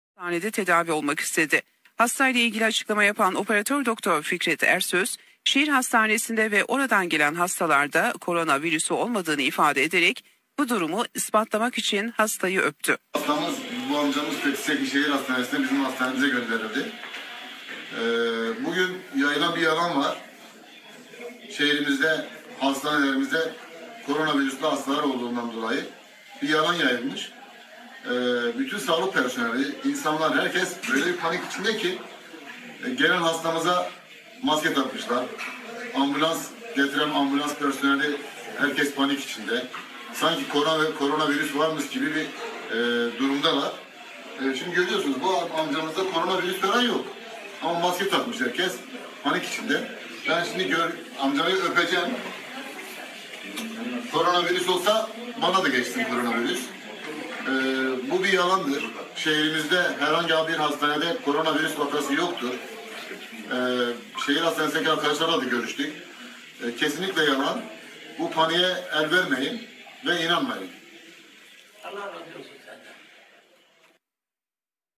Doktor Hastaların Koronavirüsü Taşımadıklarını Öperek Kanıtlıyor
Memleketten İnsan Manzaraları... Yine sadece bizde olur diyeceğimiz bir olay... Elazığ'da bir doktora hastalarının corona virüsü taşımadığını ispat etmek için tüm tıp yöntemlerini geride bırakacak bir yöntem geliştirdi: ÖPMEK... Yanaklarına (!!!)  kondurduğu öpücük ile onların bu virüsü taşımadığını belgeledi.